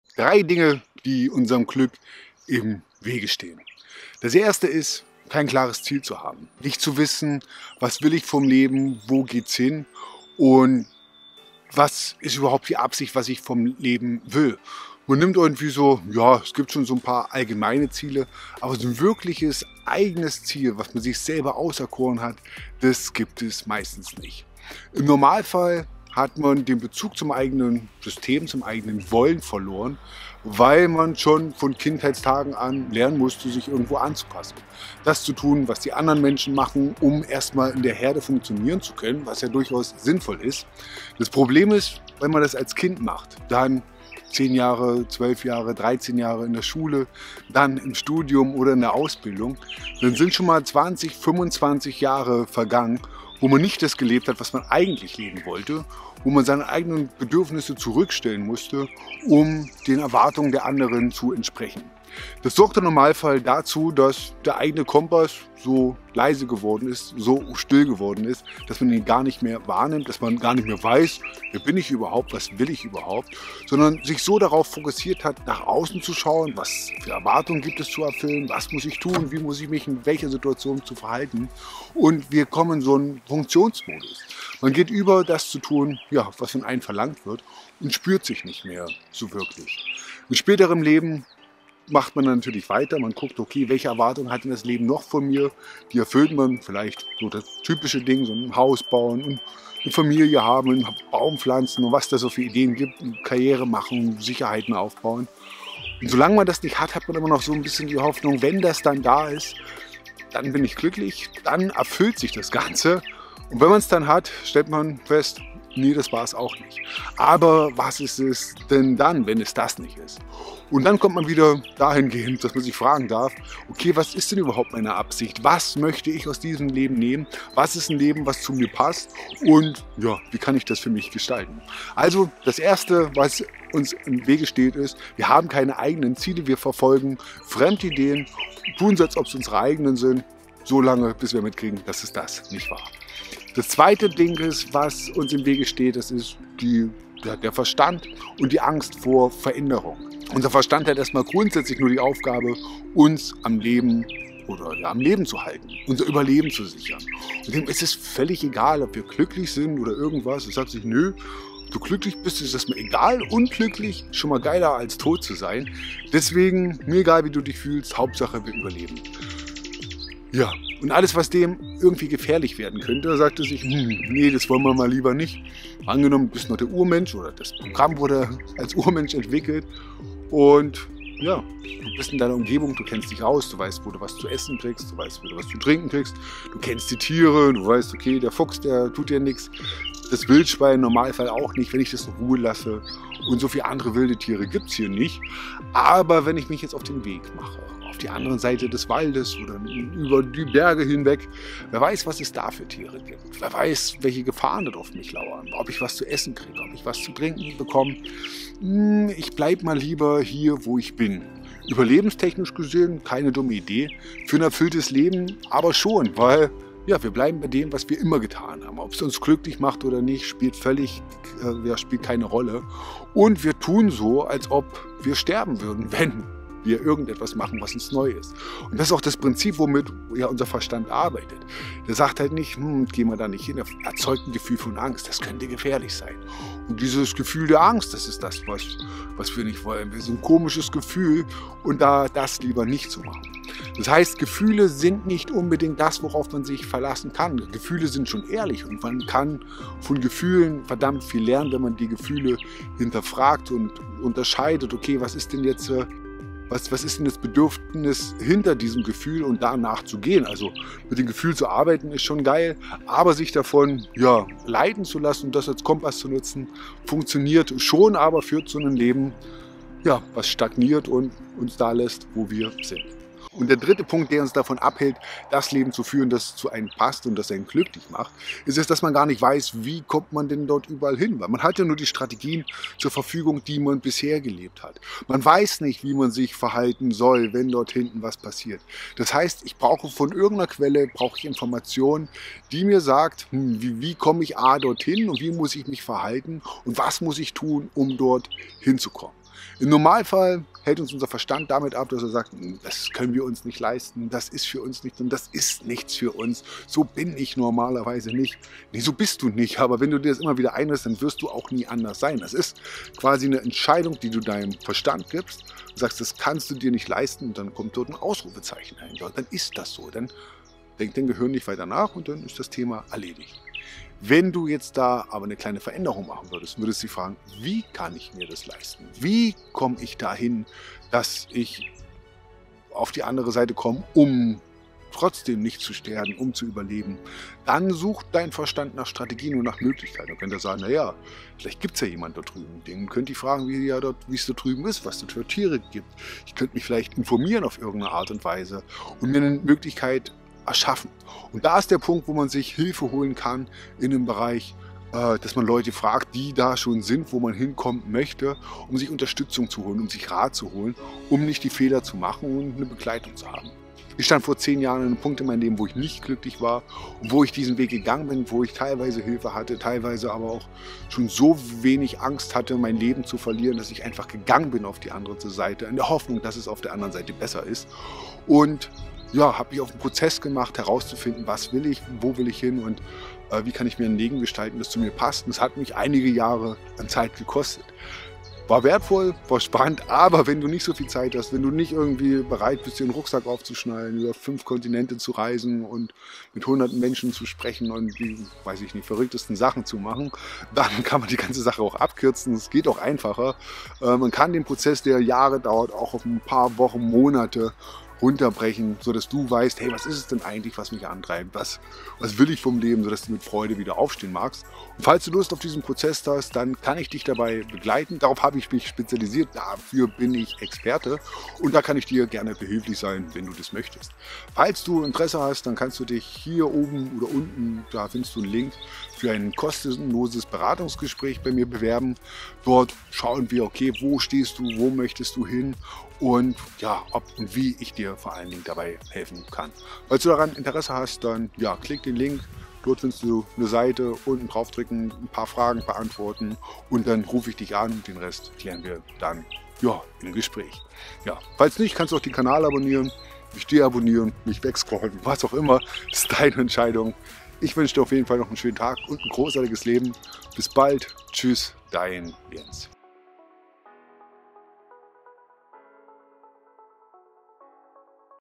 3 Dinge, die deinem Glück im Wege stehen
Freude und Erfüllung wünschen sich Viele Menschen und sind Grundvoraussetzungen für das persönliche Lebensglück. Doch warum tun wir uns so schwer? Und diesem Video erkläre ich warum Zielsetzung, Angst vor Veränderung und fehlende Vorbilder unserem Lebensglück oft im Wege stehen.